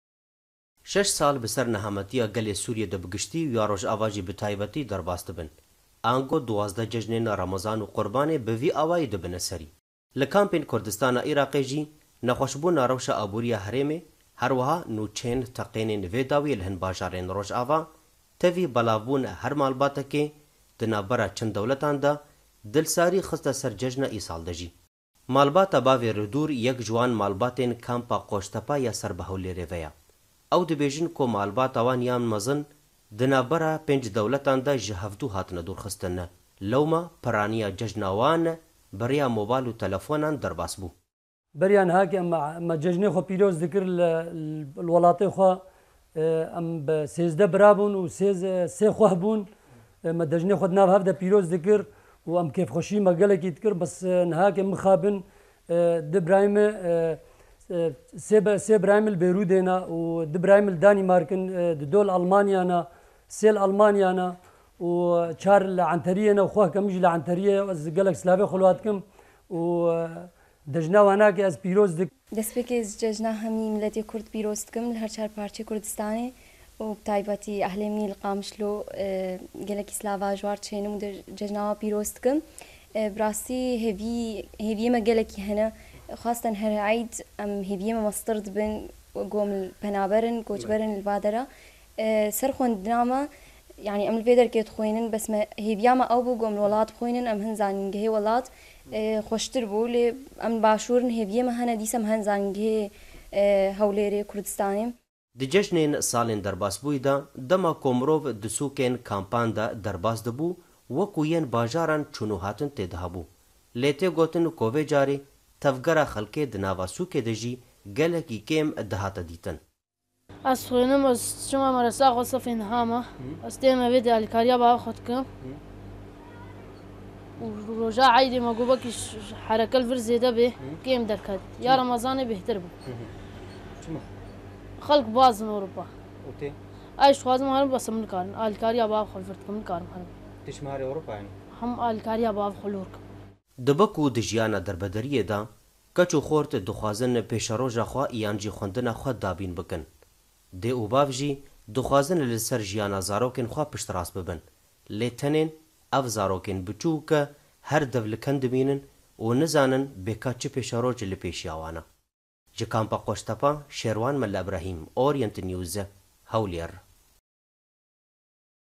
Cejna qurbanê li kampên penaberan li Herêma Kurdistanê
Li kampên herêma Kurdistanê, cejna qurbanê ya vê salê bi negermî hate derbaskirin... hin penaber jî dibêjin, sedema vê yêkê jî ewe, jiber rewşa abûrî ya nexweş ya Herêma Kurdistanê, û herweha jiber ku êdî her malbatek di nava çend dewletan de jihev hatine dûrxistin